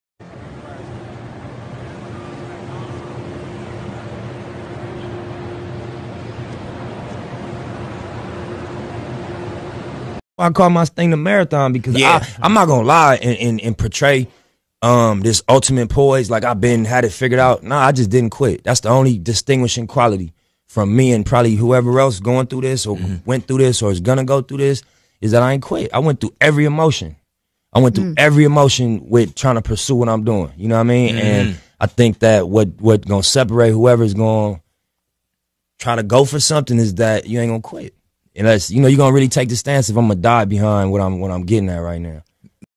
Nipsey Hussle Crime Scene 2019
Nipsey Hussle was enjoying March Madness like the rest of us Saturday night -- less than 24 hours before he was gunned down Sunday. The rapper was sitting courtside in Anaheim, CA for the Texas Tech/Gonzaga game.